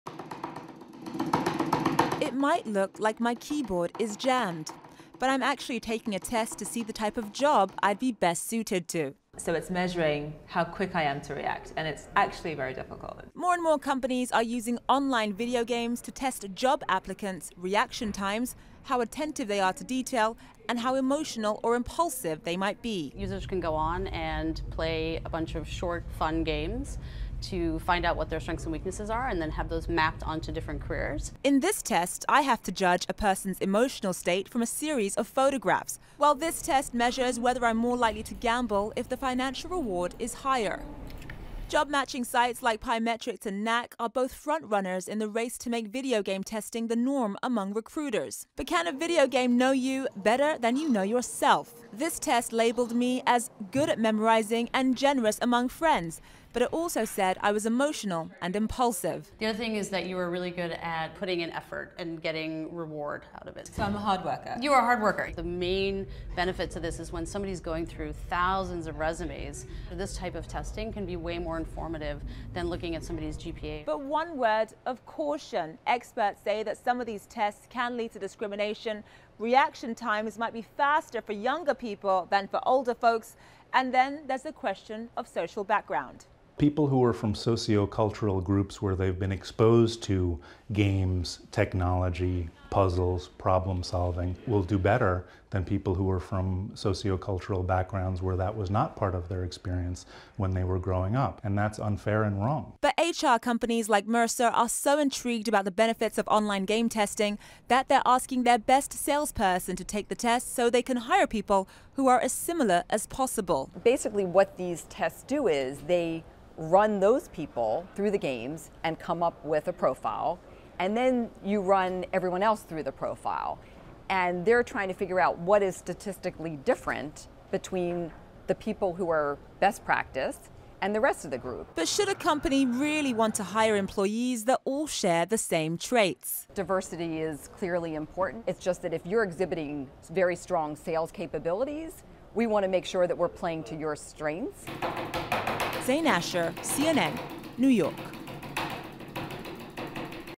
Will video games replace job interviews?
Personality testing video games are becoming the new hot high-tech tool in recruiting. But discrimination and diversity concerns could chill the hype.